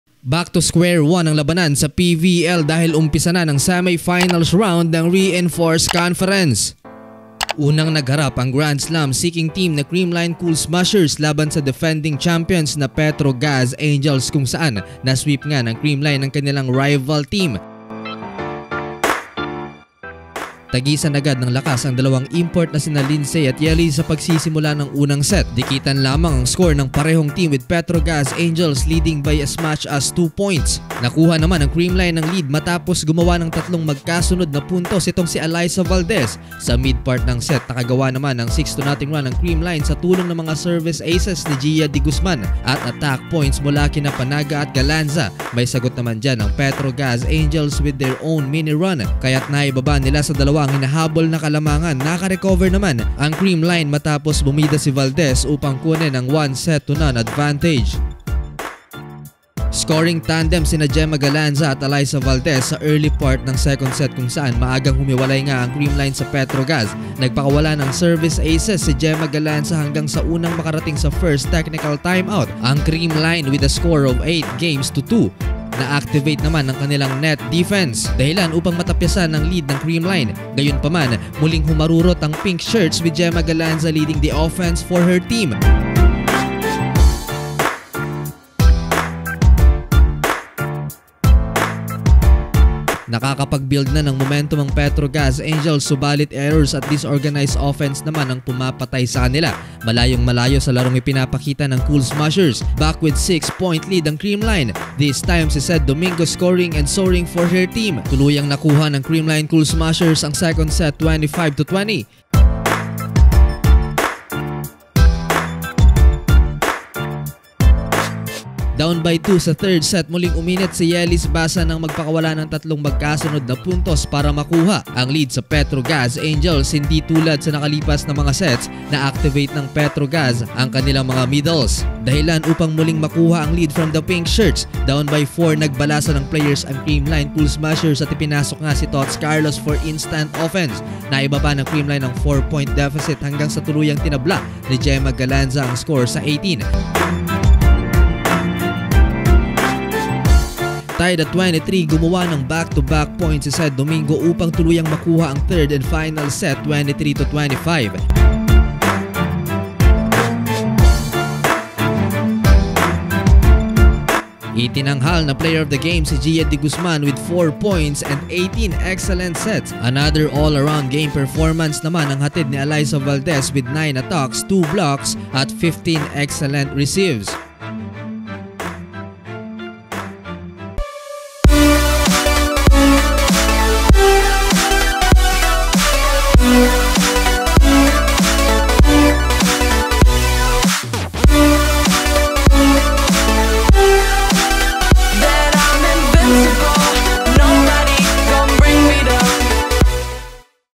Alyssa Valdez, MVP caliber na! Creamline na-sweep ang Petro Gazz sa semis! PVL Reinforced Conference
Creamline Cool Smashers vs Petro Gazz Angels